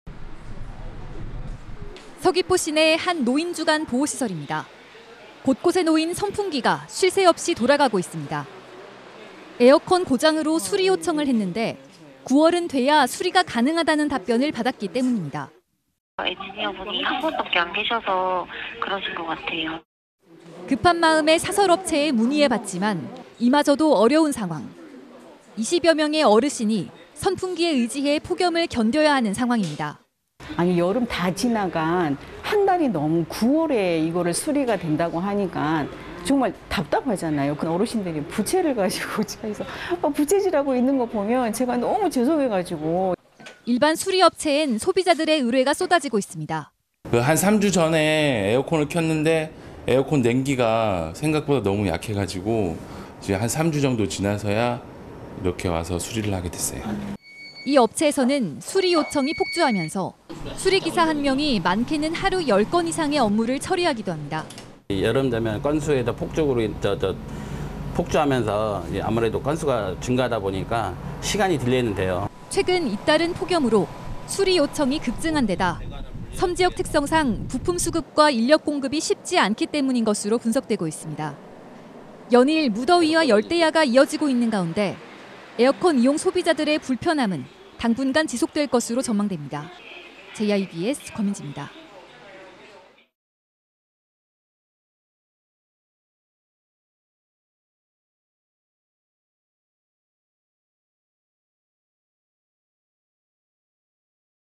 푹푹 찌는데…에어컨 수리 '하세월' / SBS /
서귀포 시내 한 노인 주간보호시설입니다.

곳곳에 놓인 선풍기가 쉴 새 없이 돌아가고 있습니다.

사흘 전 에어컨 고장으로 수리 요청을 했는데, 9월은 돼야 수리가 가능하다는 답변을 받았기 때문입니다.

[전자제품 기업 서비스센터 : 엔지니어(기술자)가 한 분밖에 안 계셔서 그런 것 같아요.]

급한 마음에 사설 업체에 문의해 봤지만, 이마저도 어려운 상황.

20여 명의 어르신이 선풍기에 의지해 폭염을 견뎌야 하는 상황입니다.

[조성미/○○ 노인주간보호센터 대표 : 여름 다 지나간 9월에 수리가 된다고 하니까 정말 답답하잖아요. 어르신들이 부채로 부채질하는 것 보면 제가 너무 죄송해서…]

일반 수리업체엔 소비자들의 의뢰가 쏟아지고 있습니다.

[김동진/에어컨 수리 요청 소비자 : 3주 전에 에어컨을 켰는데 에어컨 냉기가 생각보다 너무 약해서. 한 3주 정도 지나서야 이렇게 수리하게 됐습니다.]

이 업체선 수리 요청이 폭주하면서 수리 기사 한 명이 많게는 하루 10건 이상의 업무를 처리하기도 합니다.

[문성우/에어컨 업체 대표 : 여름 되면 (수리 신고가) 폭주하면서 아무래도 건수가 증가하다 보니까 시간이 지연돼요.]

최근 잇따른 폭염으로 수리 요청이 급증한 데다, 섬 지역 특성상 부품 수급과 인력 공급이 쉽지 않기 때문인 것으로 분석되고 있습니다.

연일 무더위와 열대야가 이어지고 있는 가운데, 에어컨 이용 소비자들의 불편함은 당분간 지속될 것으로 전망됩니다.

JIBS 권민지입니다.

(취재 : 권민지 JIBS, 영상취재 : 오일령 JIBS, 편집 : 박기덕, 제작 : 디지털뉴스편집부)